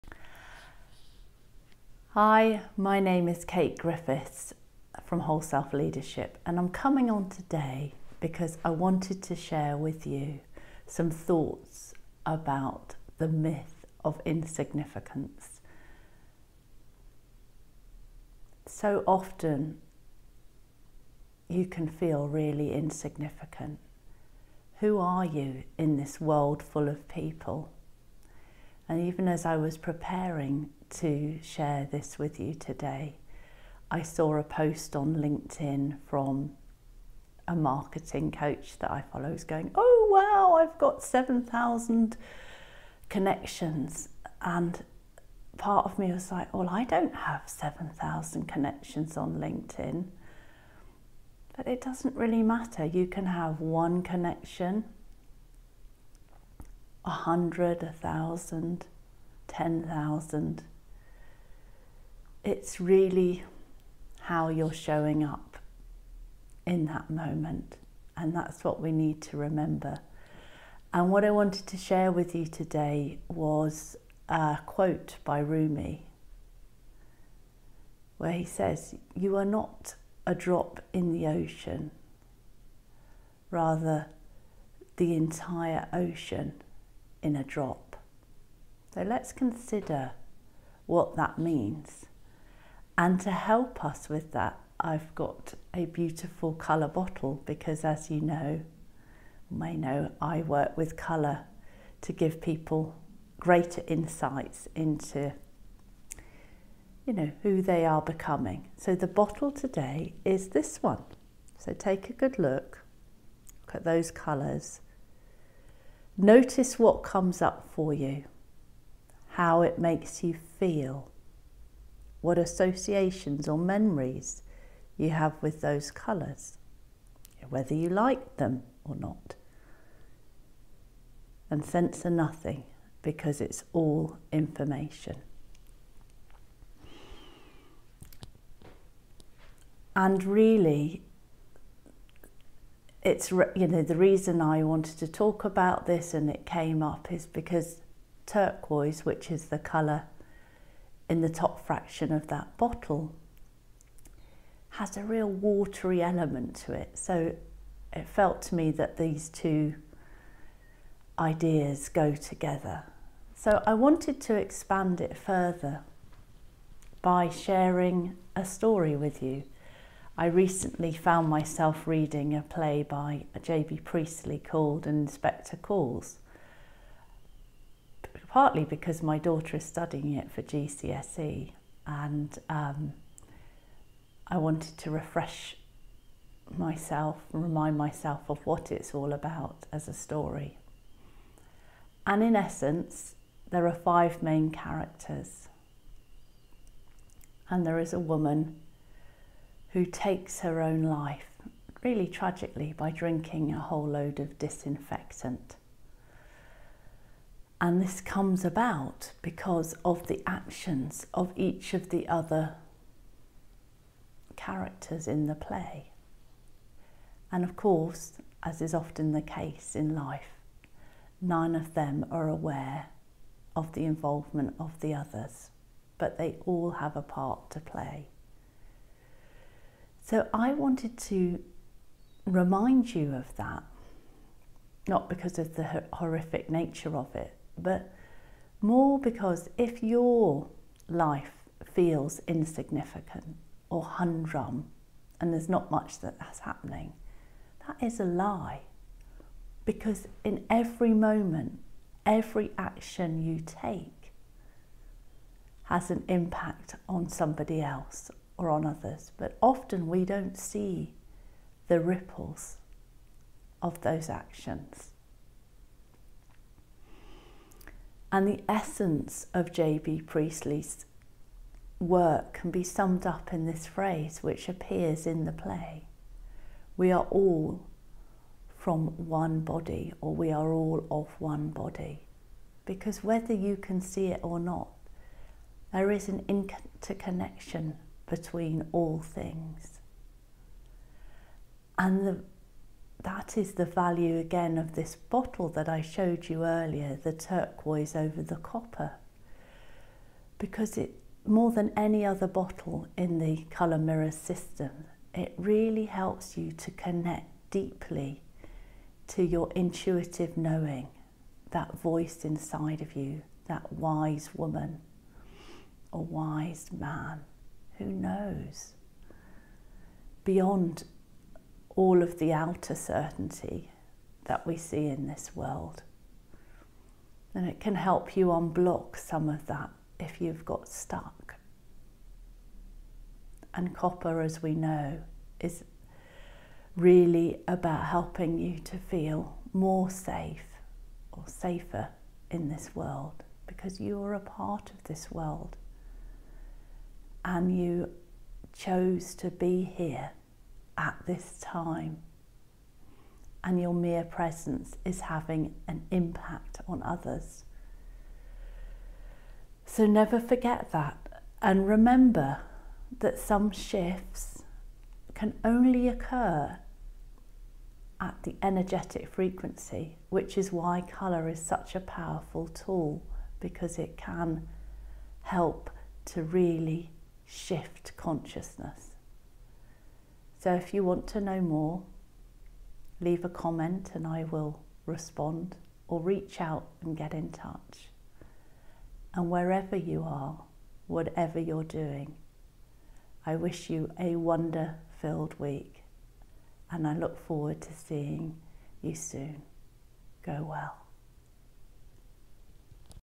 The myth of insignificance
The focus of 2021 is about finding your gold by standing in your sovereignty. This video gives you some of the keys on how to do that by unpacking the myth of insignificance.